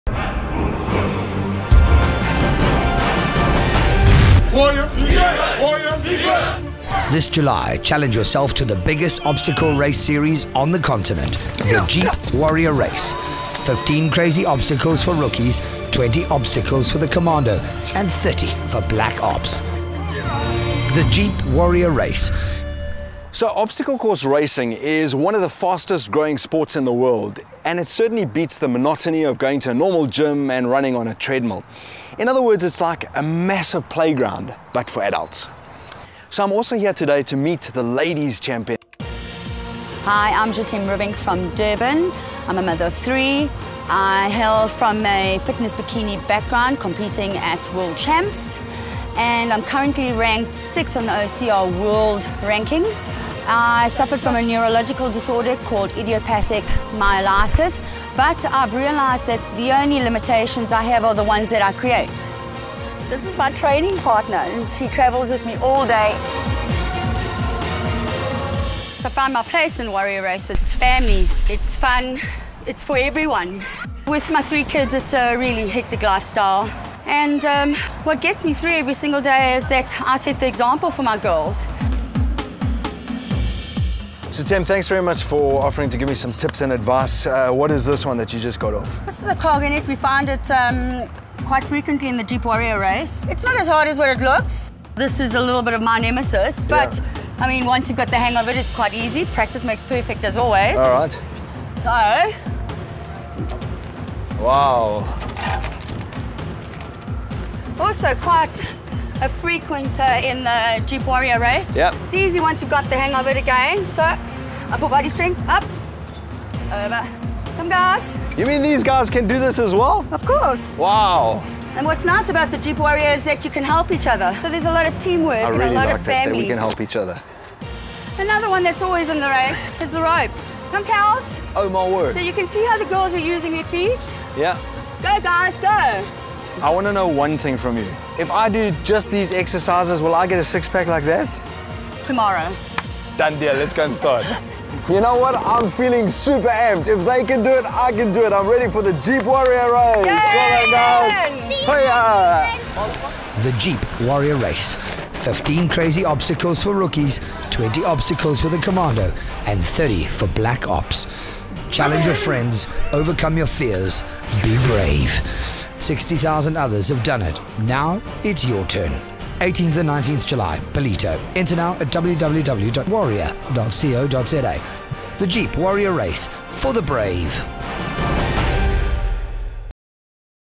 BlackOps Jeep Warrior ladies champ inspires rookie DJ
East Coast Radio presenter Damon Beard has been challenged by his producer, Rory Petzer, to take part in the Jeep Warrior race next weekend (18 and 19 July) at Ballito. And Damon's embraced the challenge with both hands. And he's getting advice from the best of the best. Here, 4 X Warrior Black Ops Elite Ladies Winner – Jet’aime Ribbink, shares a few pointers.